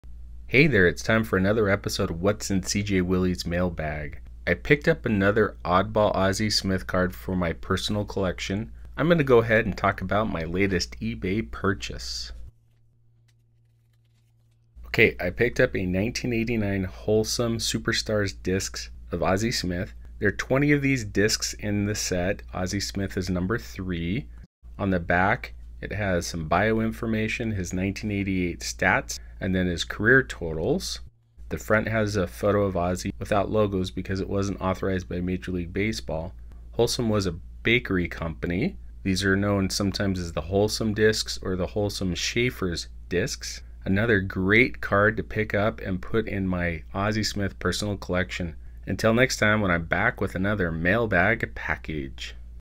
Ozzie Smith Holsum Disc Card - Cracking Mailbag Packages 37
1989 Holsum Disc Card
Ozzie Smith
Baseball
Opening